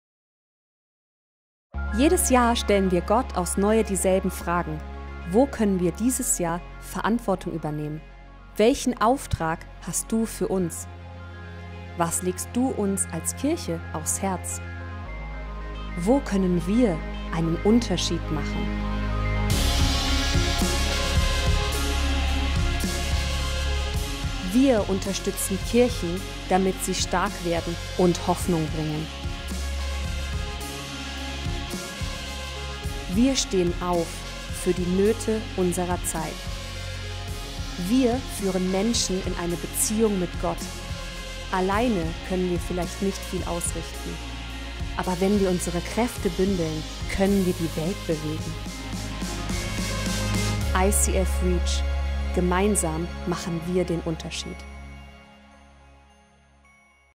Reach Heartbeat 2017
REACH HEARTBEAT
Wir sind begeistert und dankbar, dass sich unsere Kirche jedes Jahr auch außerhalb ihrer eigenen vier Wände engagiert. Wir merken: Gott segnet uns mit vielfältigen Ressourcen. Diese Großzügigkeit wollen wir weitergeben! So können wir mit vereinten Kräften einen Unterschied in unserer Nachbarschaft, unserem Land und in der Welt machen.
Die REACH Projekte entstehen aus dem tiefen Wunsch, uns in Gottes Träume einzuklinken. Wenn er fragt, wer mit ihm zusammenarbeiten möchte, wollen wir rufen: „Hier! Wir! Mach das mit uns!“
Deshalb legen wir auch dieses Jahr für Projekte in Stadt, Land und darüber hinaus zusammen. Egal ob Kirchengründungen, Events, Medienarbeit oder Umbauten, eines haben diese Projekte gemeinsam: sie haben das Ziel, dass Menschen Jesus kennenlernen. Am Ende des Tages ist das der Antrieb für alles was wir tun.